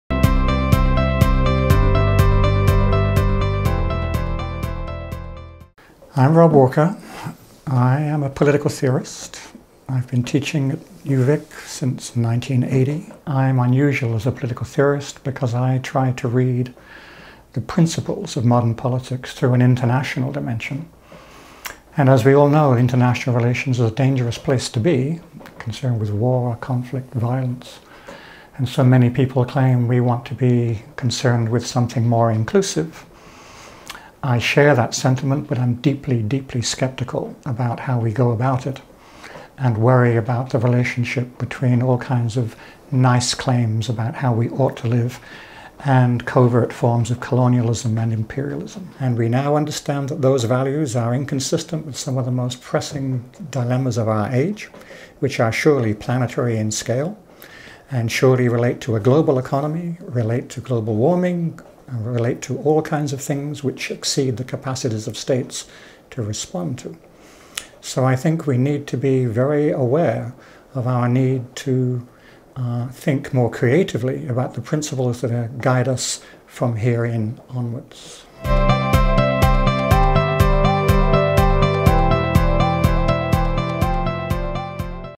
Faces of UVic Research: Rob Walker
Dr. Rob Walker is a faculty member of the Department of Political Science at the University of Victoria Faculty of Social Sciences. In this video, Rob discusses his work as a political theorist.